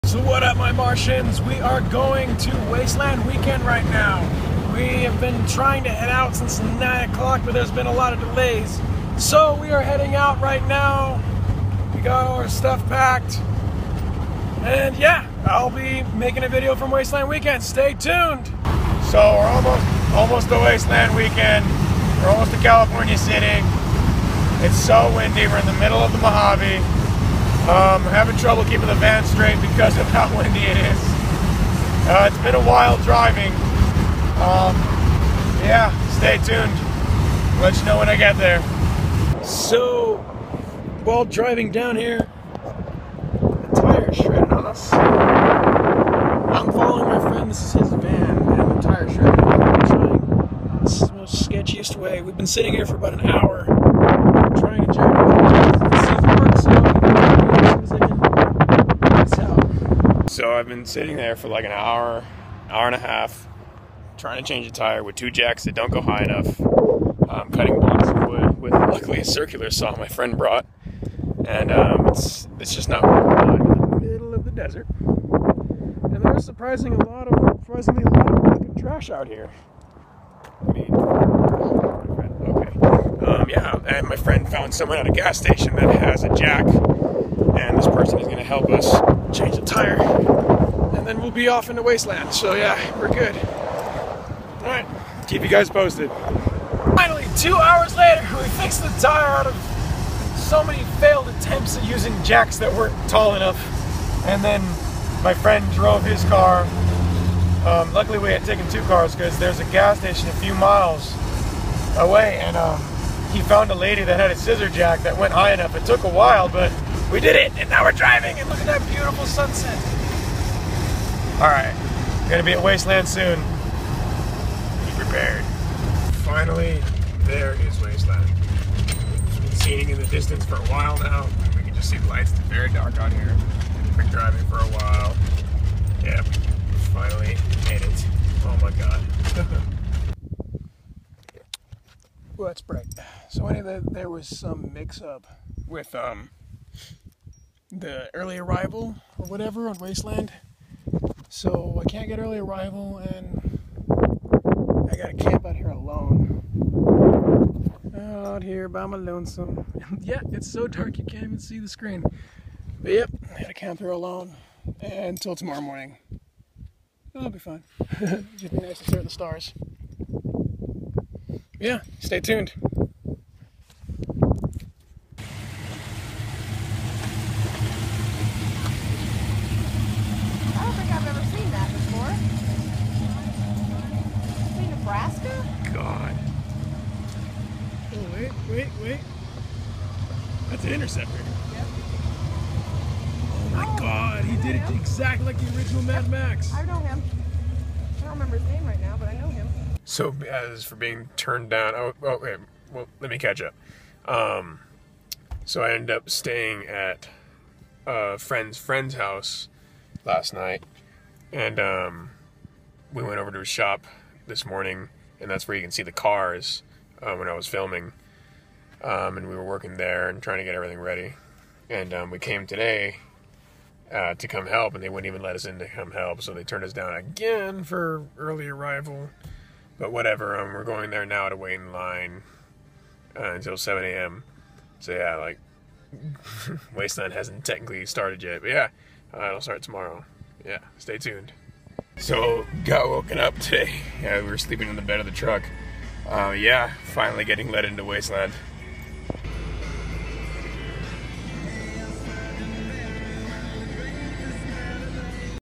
WASTELAND WEEKEND 2015 part 1
My  first wasteland weekend in a 3 part series

my phones memory filled up throughout the week, there was just so much to capture.